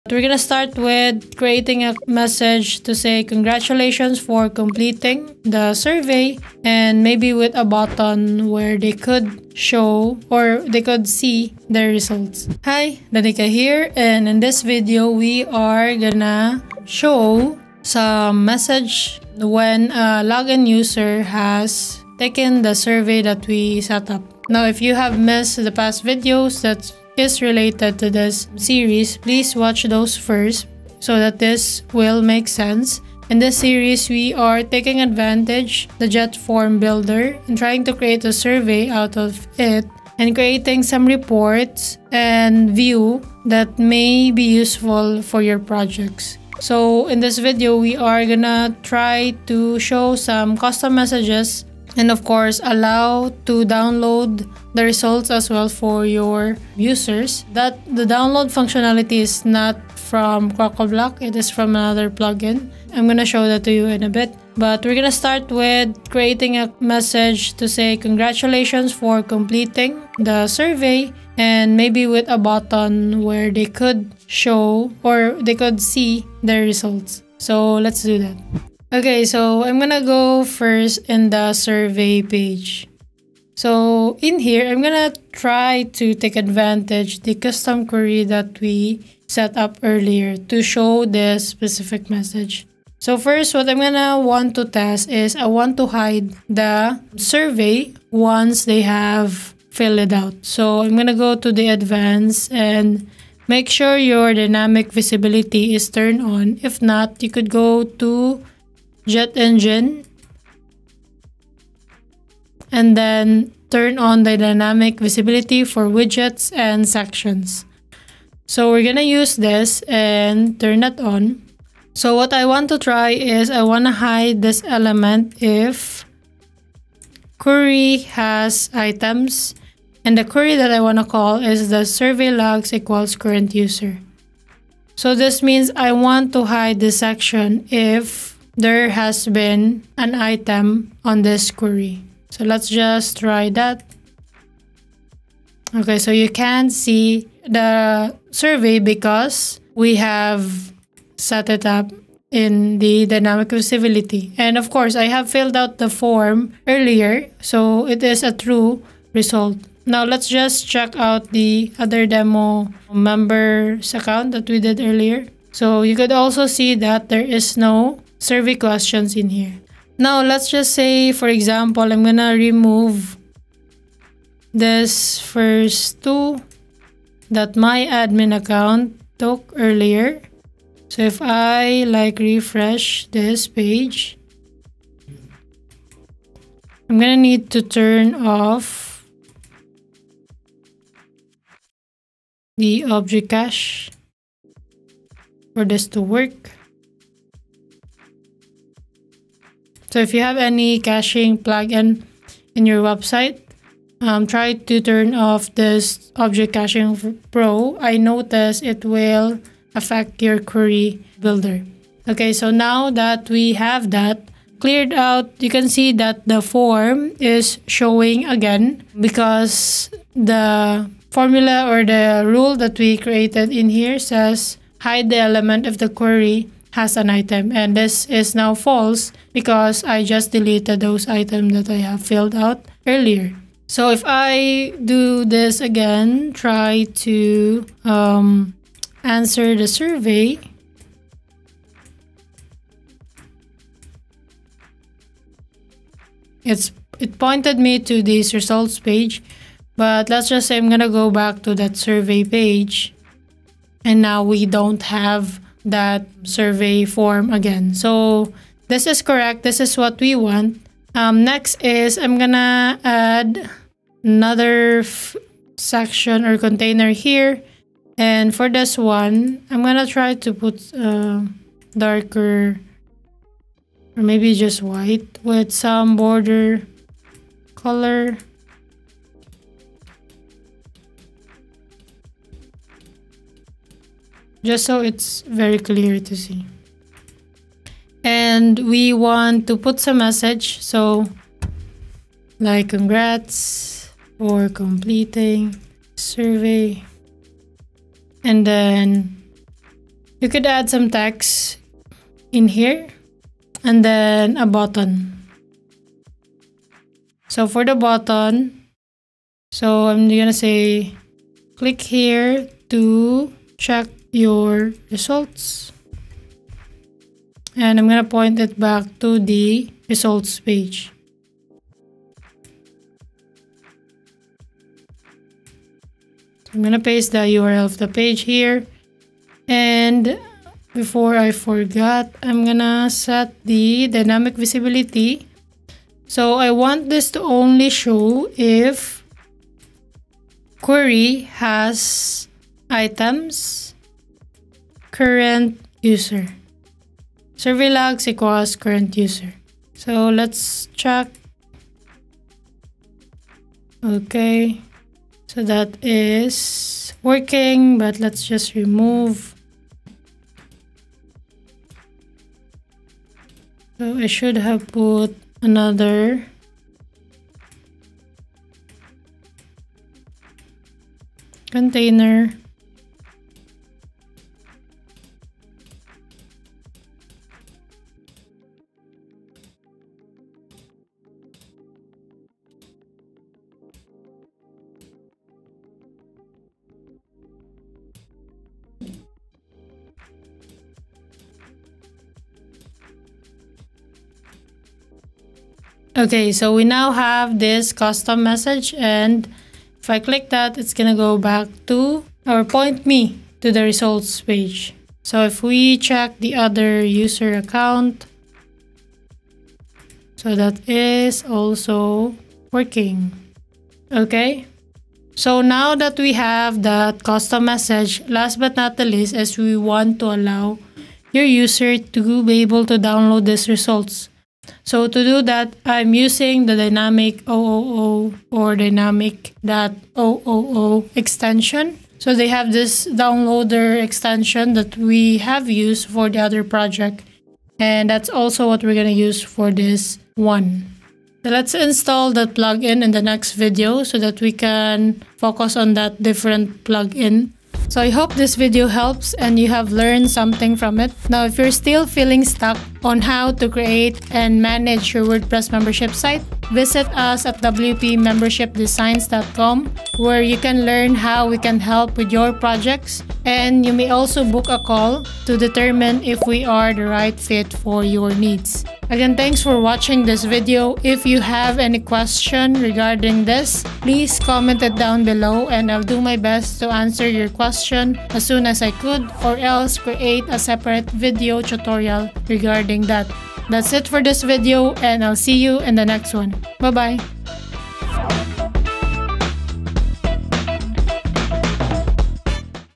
How to Add Custom Messages for Completed Surveys in WordPress | JetForm Builder
In this video, I will show you how to add custom messages and a download button when users complete a survey on your WordPress membership site. We will be using JetForm Builder and Dynamic.oo extension to create a unique user experience after a user completes a survey.

What You’ll Learn:

• How to hide the survey form after a user completes it using Dynamic Visibility.
• Setting up a "Congratulations" message and a button that leads users to their survey results.
• Adding a download button for users to download their survey results.
•Troubleshooting issues with cache plugins that might affect the survey visibility.

This tutorial will help you create a seamless experience for your members, allowing them to easily view or download their survey results after completion.

⏰ Timestamps:
00:00 – Introduction to Customizing Survey Results
00:35 – Hiding the Survey Form After Submission
01:10 – Adding Custom "Congratulations" Message
02:00 – Creating a "See Results" Button
03:00 – Setting Dynamic Visibility for the Message
04:00 – Troubleshooting Cache Plugin Issues
05:00 – Downloading Survey Results Using Dynamic.oo
06:00 – Final Thoughts & Next Steps

I'll do my best to help and may create more videos based on your feedback.